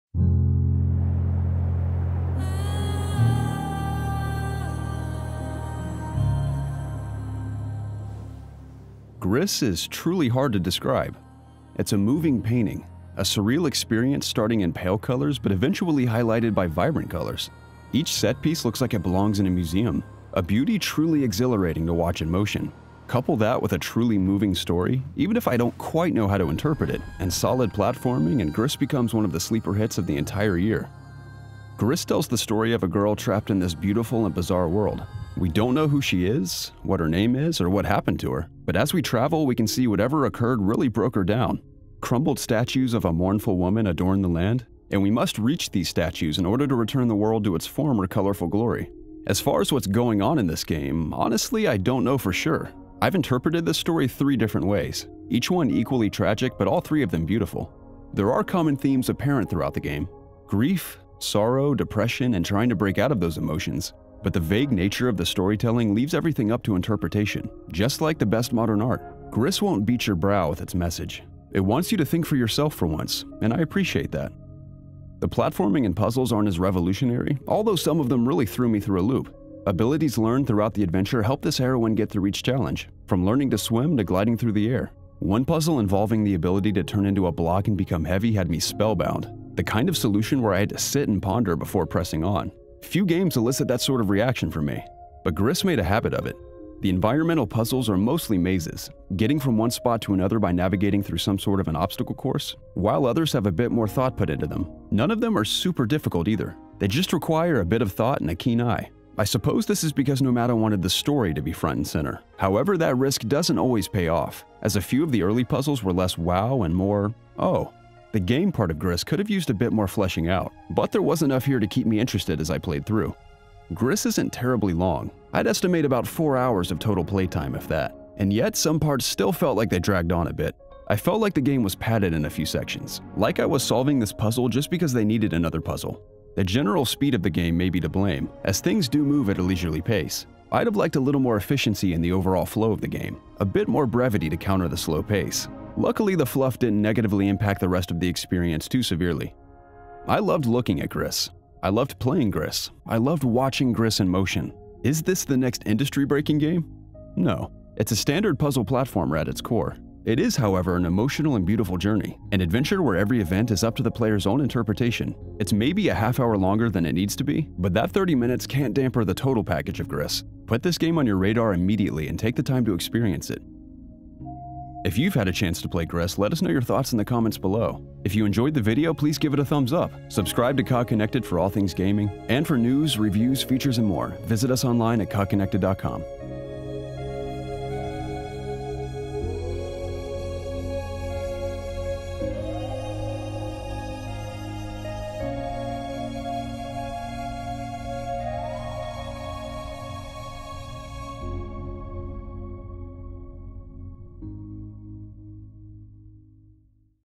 GRIS Review - The Art of Moving Pictures
I loved looking at GRIS. I loved playing GRIS. I loved watching GRIS in motion. Is this the next industry breaking game? No, it’s a standard puzzle platformer at its core. It is however an emotional and beautiful journey, an adventure where every event is up to the player’s own interpretation.